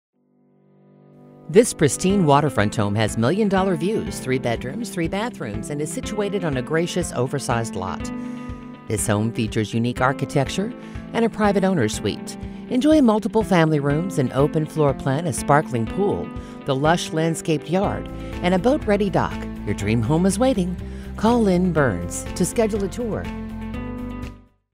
944 Apollo Beach Boulevard Apollo Beach, FL | ColdwellBankerHomes.com
944 Apollo Beach Boulevard, Apollo Beach, FL

This pristine waterfront home with million-dollar views is ready for even the most discerning buyer. Situated on a gracious, oversized lot, this multi-level three bedroom, three bathroom home with unique architectural features does not disappoint! A private owners suite, multiple family rooms, open